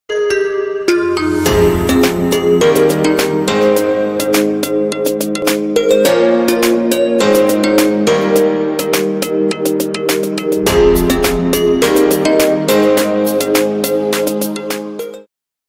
Jadi santri itu seru🤗
kapan lagi mau dekat dengan Allah, selagi Allah masih memberikan kita hidup maka perbanyaklah mengingat beliau, dan dekat dengan rumah Allah SWT